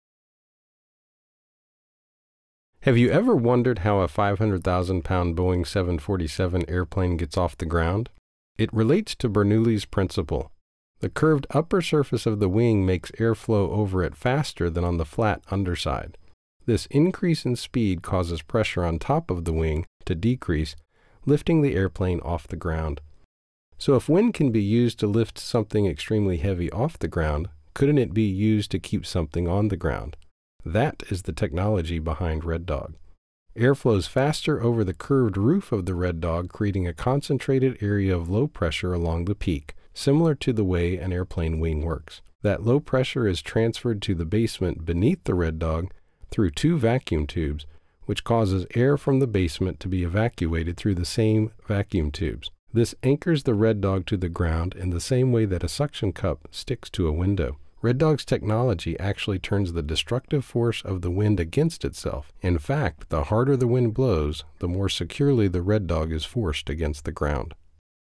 Red Dog Protects Mobile Shelters - Bernoulli's Principle
Here is an explanation of how Red Dog Mobile Shelters uses Bernoulli's Principle to keep their mobile shelters safe and secured firmly to the ground.